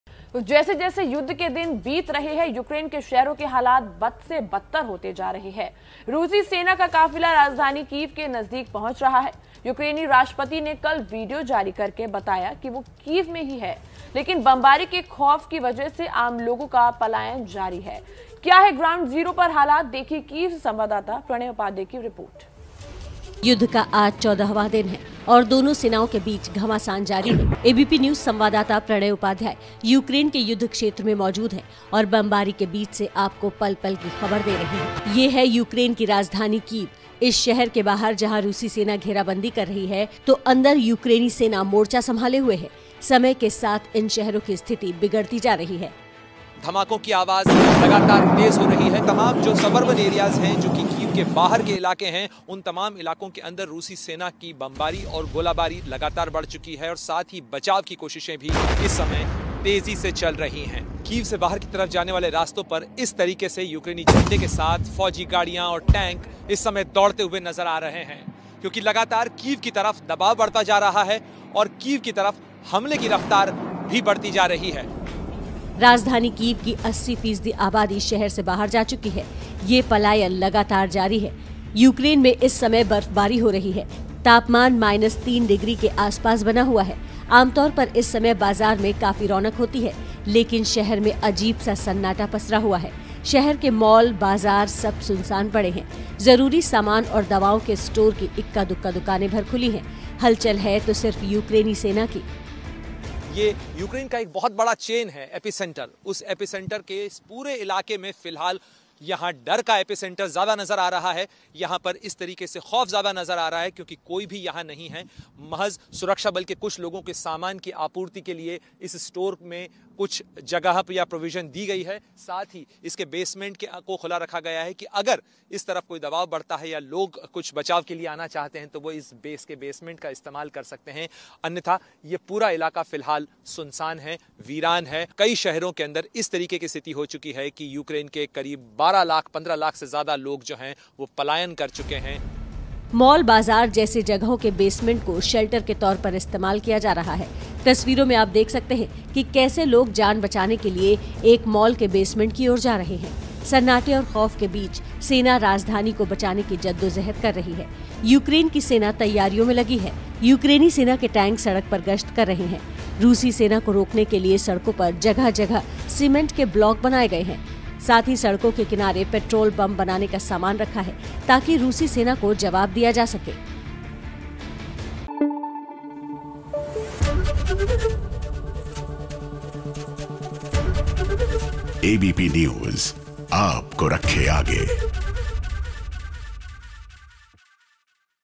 यूक्रेन की राजधानी कीव से लोगों का पलायन जारी, देखिए ताजा हालात | Russia Ukraine War Ground Report
जैसे-जैसे युद्ध के दिन बीत रहे हैं.. यूक्रेन के शहरों के हालात बदतर हो रहे हैे.. रूसी सेना का काफिला राजधानी कीव के नजदीक पहुंच रहा है । यूक्रेनी राष्ट्रपति ने कल वीडियो जारी करके बताया कि वे कीव में ही हैं.. लेकिन बमबारी के खौफ की वजह से आम लोगों का पलायन जारी है.. क्या हैं ग्राउंड जीरो पर हालात.. देखिए कीव से संवाददाता प्रणय उपाध्याय की रिपोर्ट.